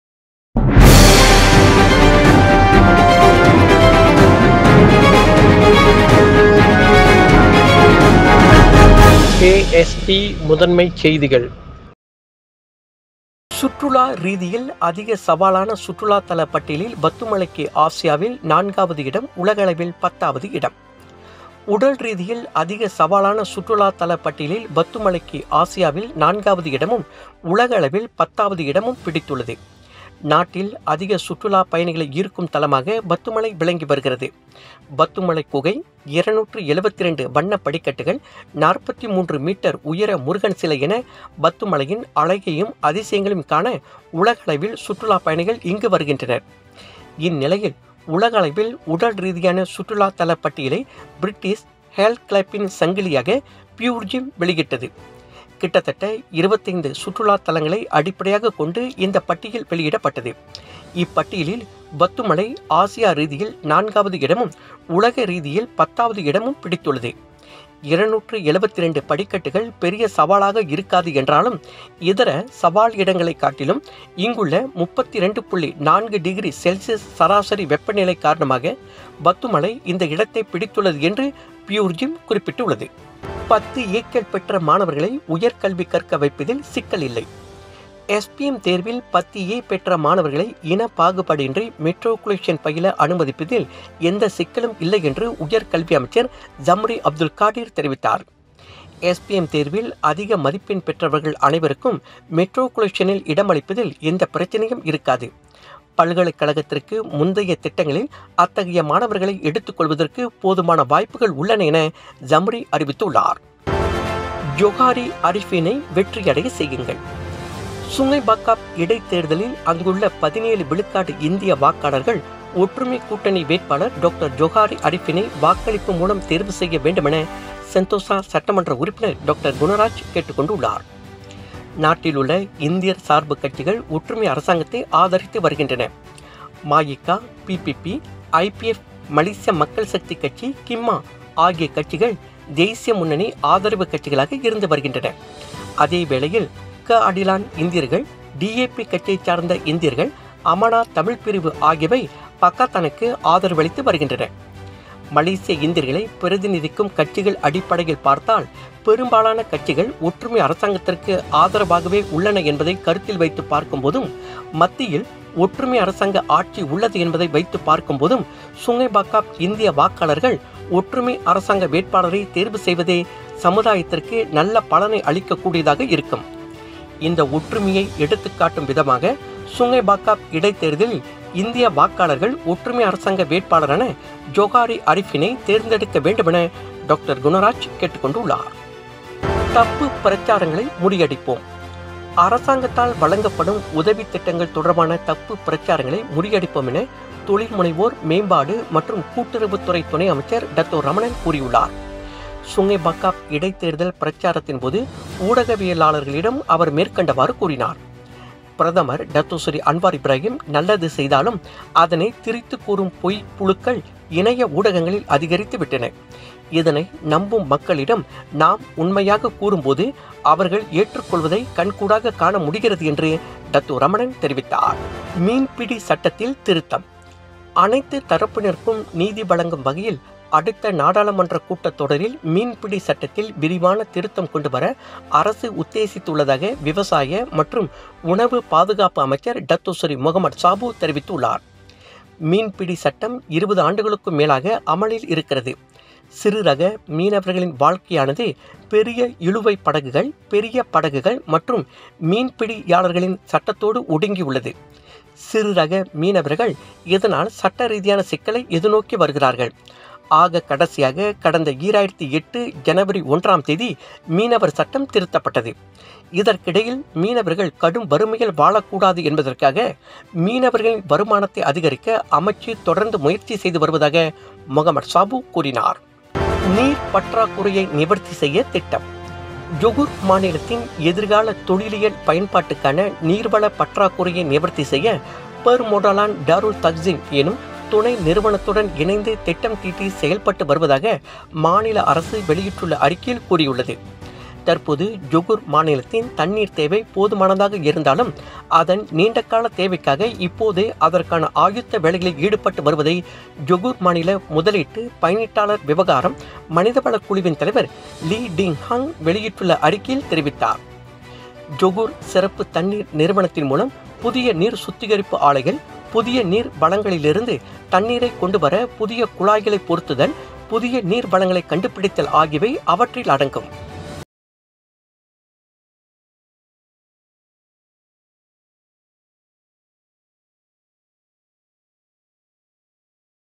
🔴 WATCH NOW | AST News | பத்துமலைக்கு கிடைத்த பெருமை! ஆசியாவில் 4ஆவது இடம்!
✪  10 ஏக்கள் பெற்ற மாணவர்களை உயர் கல்வி கற்க வைப்பதில் சிக்கல் இல்லை!

✪  நீர் பற்றாக்குறையை நிவர்த்தி செய்ய திட்டம்!

✪ ஜொஹாரி அரிபின்னை வெற்றியடையச் செய்யுங்கள்!